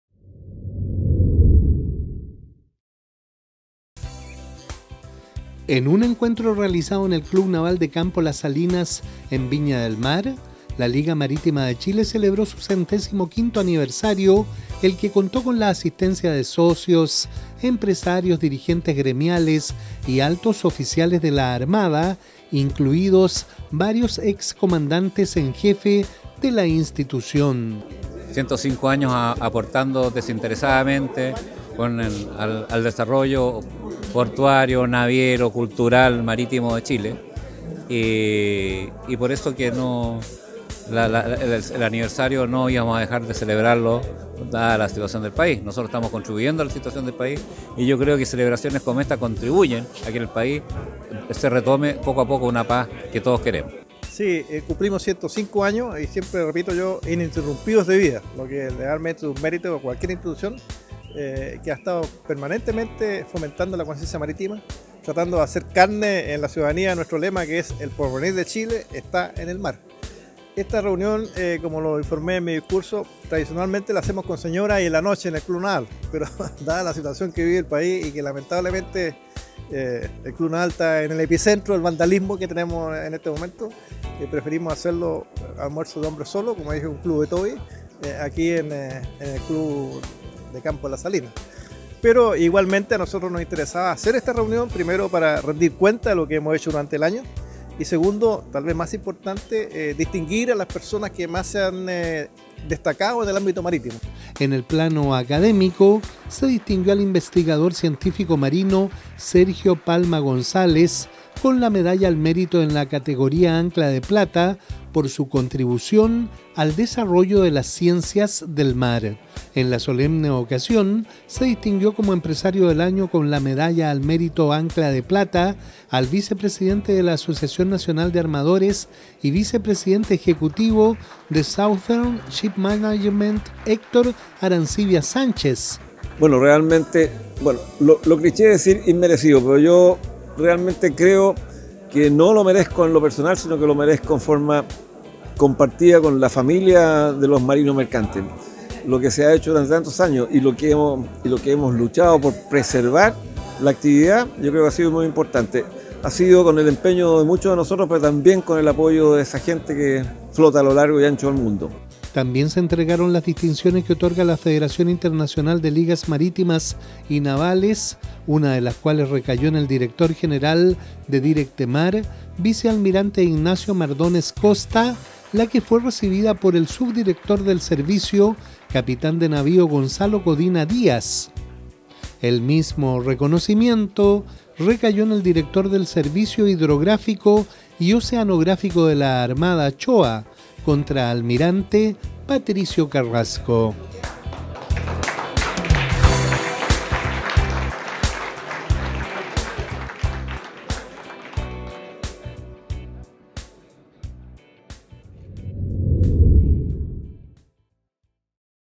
105º Aniversario Liga Marítima de Chile
Vice presidente de armadores, Héctor Arancibia, es elegido Empresario  del Año por la Liga Marítima. Como científico destacado fue galardonado el investigador Sergio Palma González. Por su parte, la Federación Internacional de Ligas Marítimas, condecoró al vicealmirante Ignacio Mardones y al contraalmirante Patricio Carrasco.